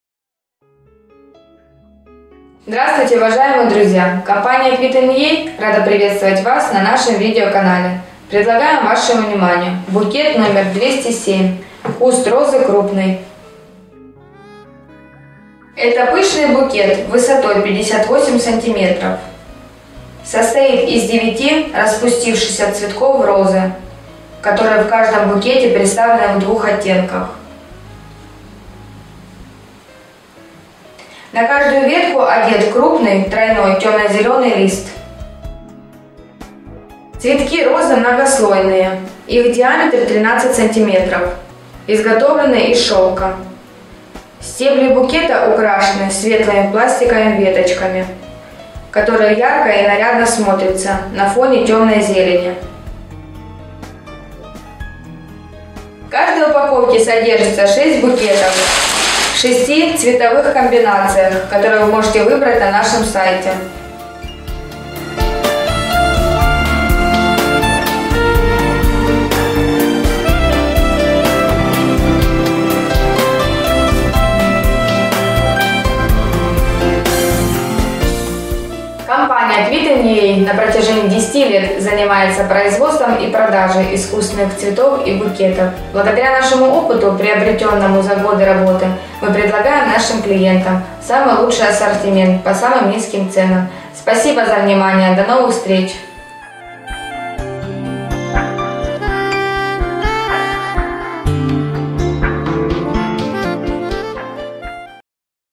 Лучшие букеты от Kvitu.in.ua! № 207 Букет
Изысканный букет представляет собой головокружительный букет из пышных роз двух различных расцветок. Палитра подобрана таким образом, что каждый из них эффектно дополняет другой. Букет состоит из стебля 58 см и 9 цветочных голов из натурального шелка. Кроме того, он украшен бурной зеленью и декоративными добавками, что придает еще большей пышности. В магазине есть 6 вариантов цветового исполнения. При заказе оптовой упаковки, вы получаете все 6 образцов с доставкой в любой город Украины.